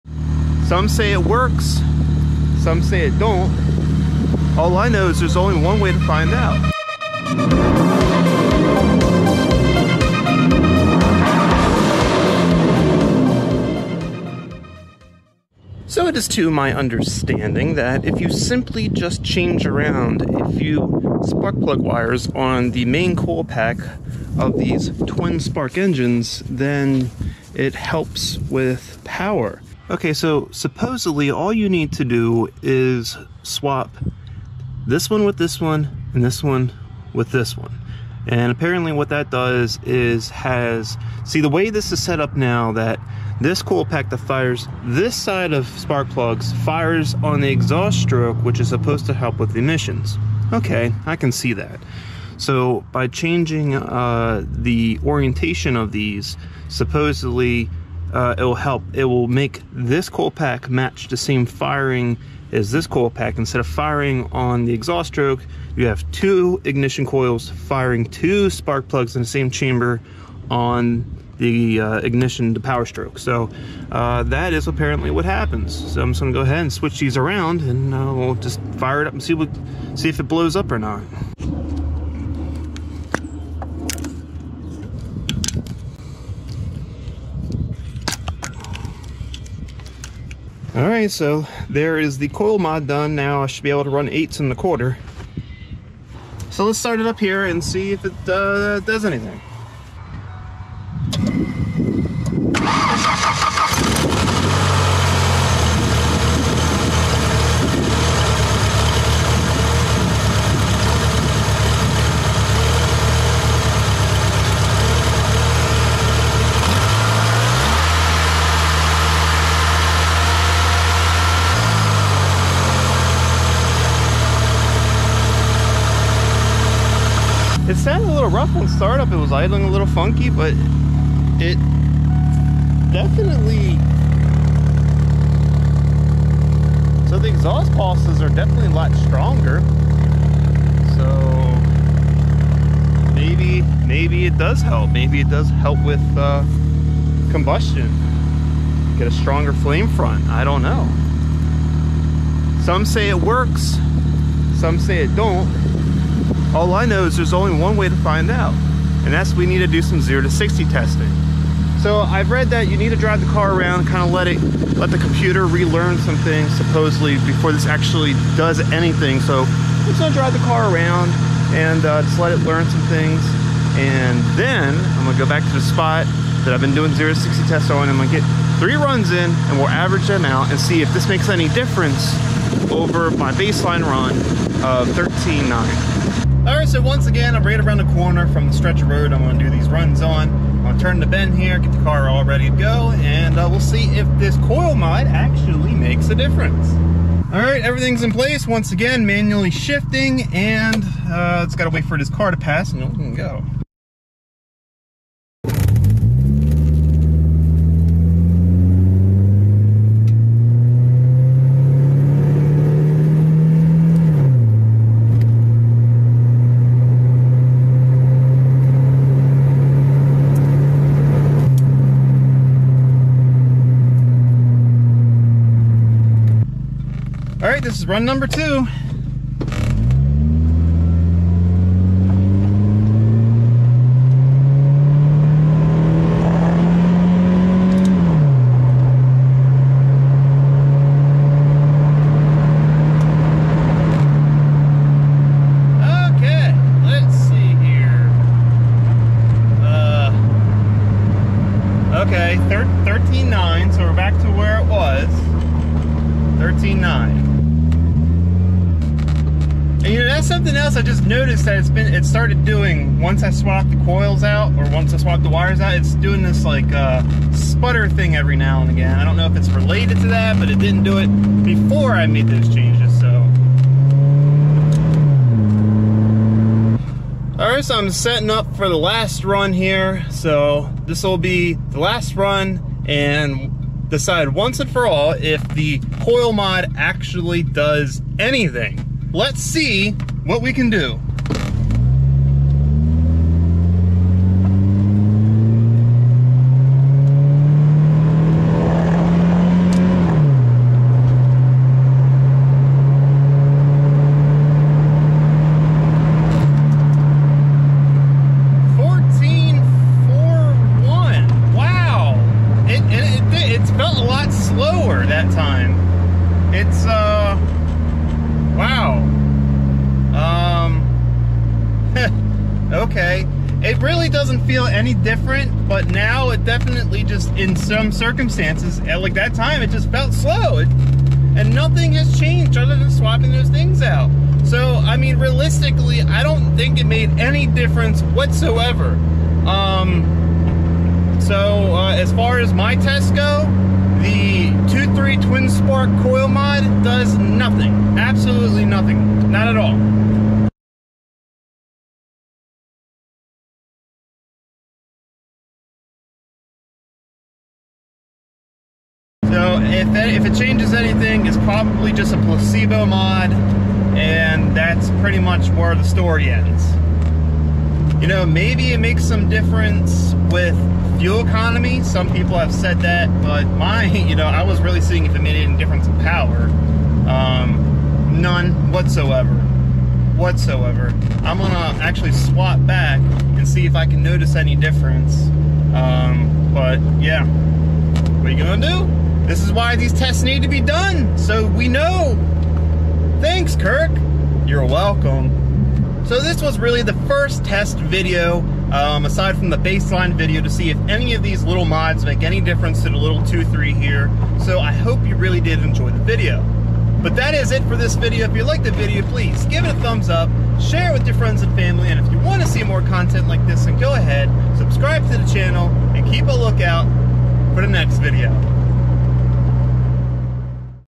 Ford Twin Spark 2.3L SOHC Coil Pack Mod Tested 0 To 60 MPH - Science Bro
In this video, I test the effectiveness of a popular free modification for Ford's 2.3L SOHC Engine. I utilize my trusty Dragy to log three 0 to 60 MPH runs. The average time of the three runs is then calculated.

🔹Visit the Karz Kreated Blog

Car Meets @karz_kreated